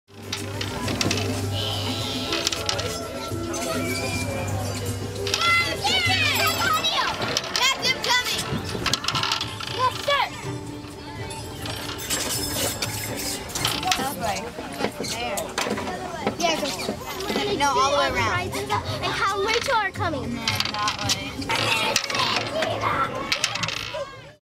Ball Maze Panel - PlayBooster® - Landscape Structures
The PlayBooster® Ball Maze Panel is never the same game twice. Fun for one or more kids and promotes creative play. The Ball Maze Panel provides lots of playground fun for kids ages 5-12.